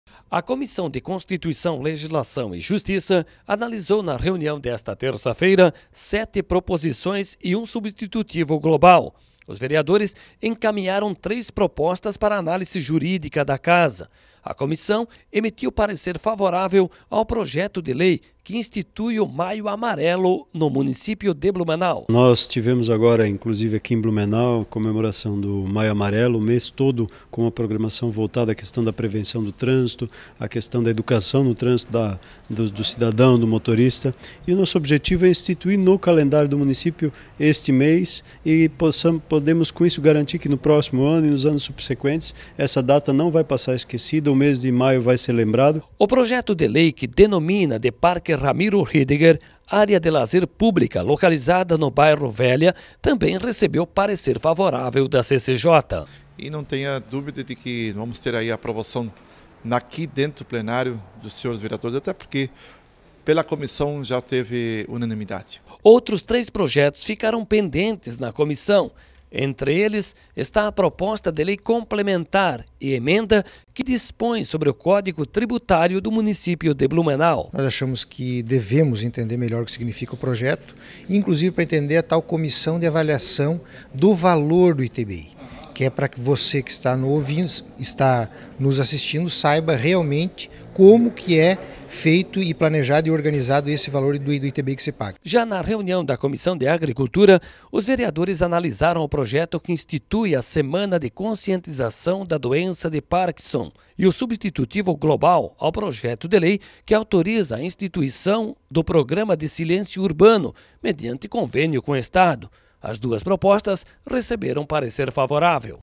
CCJ analisa sete projetos de lei e um substitutivo global
A comissão de constituição, legislação e justiça analisou, na reunião desta terça feira, sete proposições e um substitutivo global. Os vereadores encaminharam três propostas para análise jurídica da casa. A comissão emitiu parecer favorável ao projeto de lei que institui o Maio Amarelo no município de Blumenau